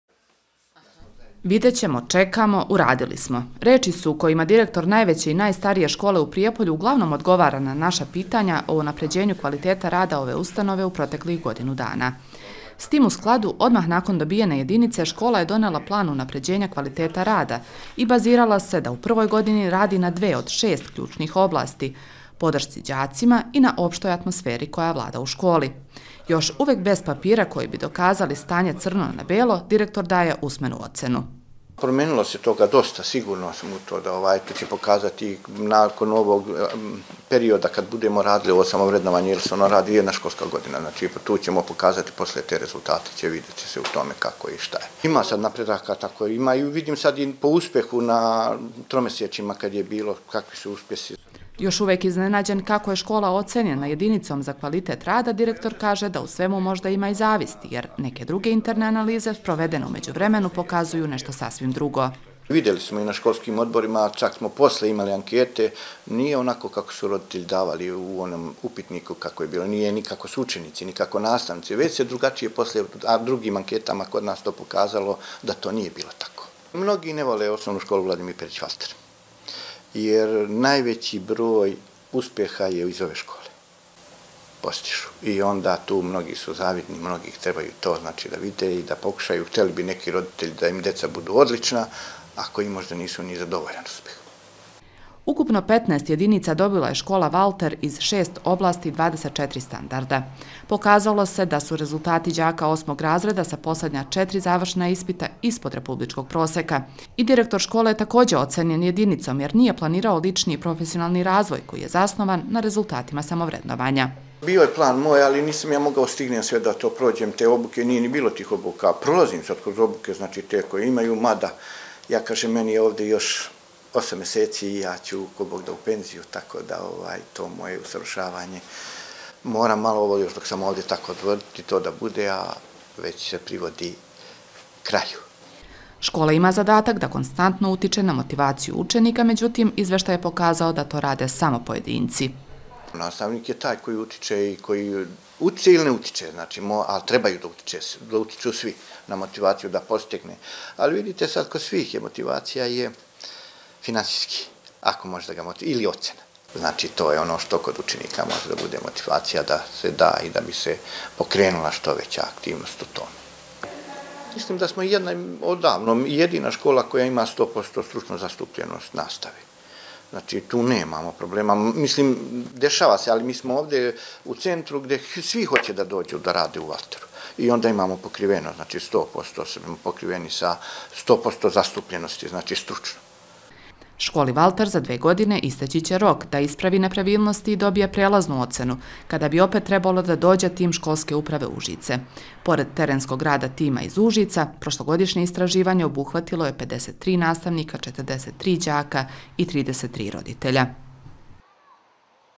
Godinu dana od jedinice OŠ „Vladimir Perić Valter“
Prošlo je godinu dana od kada je Osnovna škola „Vladimir Perić Valter“ ocenjena jedinicom za opšti kvalitet rada, prilikom spoljašnjeg vredovanja tima Školske uprave Užice. Direktora škole Milinka Mićevića pitali smo šta je urađeno u međuvremenu, a što bi moglo da utiče na bolju ocenu prilikom narednog ocenjivanja škole?